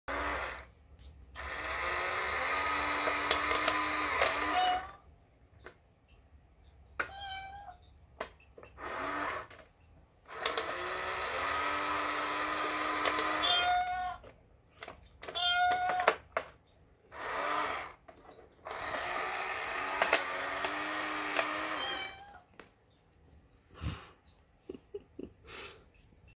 Miller and Oreo playing with Japanese coin bank
Miller shows Oreo how to work the Japanese coin bank